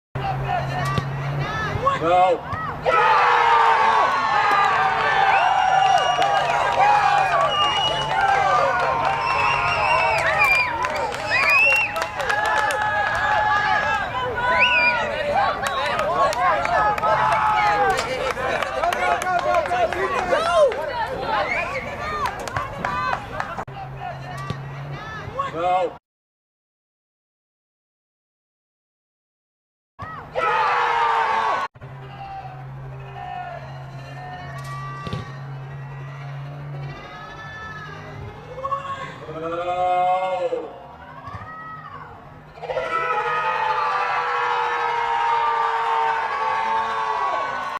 Erik scores against Bethesda in Maryland
Erik's game winning playoff goal vs Bethesda 2000 Pre Academy